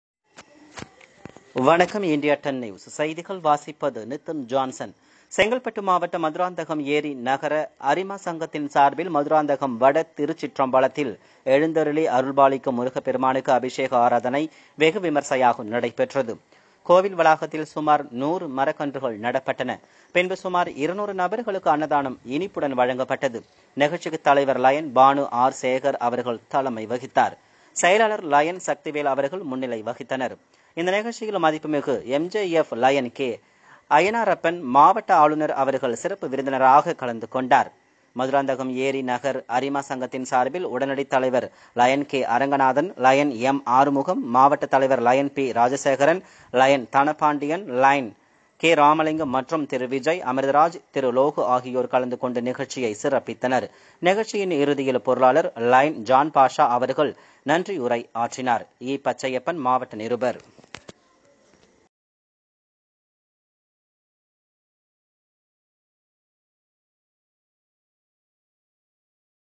சுமார் 200 நபர்களுக்கு அன்னதானம் இனிப்புடன் வழங்கப்பட்டது
செங்கல்பட்டு மாவட்டம்  மதுராந்தகம் ஏரி நகர அரிமா சங்கத்தின் சார்பில்  மதுராந்தகம் வட திருச்சிற்றம்பலத்தில் எழுந்தருளி அருள்பாலிக்கும் முருகப் பெருமானுக்கு அபிஷேக ஆராதனை வெகு விமர்சையாக நடைபெற்றது கோவில் வளாகத்தில் சுமார் 100மரக்கன்றுகள் நடப்பட்டன பின்பு சுமார் 200 நபர்களுக்கு அன்னதானம் இனிப்புடன் வழங்கப்பட்டது நிகழ்ச்சிக்கு தலைவர் லயன் பானு ஆர் சேகர் அவர்கள் தலைமை வகித்தார் செயலாளர் லயன் சக்திவேல் அவர்கள் முன்னிலை வகித்தனர் இந்த நிகழ்ச்சியில் மதிப்புமிகு MJF Lion K.  ஐயனாரப்பன்.மாவட்ட ஆளுநர் அவர்கள் சிறப்பு விருந்தினராக கலந்து கொண்டார் மதுராந்தகம் ஏரி நகர் அரிமா சங்கத்தின் சார்பில் உடனடி தலைவர் லயன்.கே. அரங்கநாதன் லயன்.எம். ஆறுமுகம் மாவட்ட தலைவர்.லயன் பி. ராஜசேகரன். lion தனபாண்டியன் லைன் கே.ராமலிங்கம் மற்றும் திரு.விஜய். அமிர்தராஜ் திரு.லோகு ஆகியோர் கலந்துகொண்டு நிகழ்ச்சியை சிறப்பித்தனர் நிகழ்ச்சியின் இறுதியில் பொருளாளர். லைன் ஜான் பாஷா.அவர்கள் நன்றியுரை ஆற்றினார்.E .பச்சையப்பன்.மாவட்ட நிருபர்.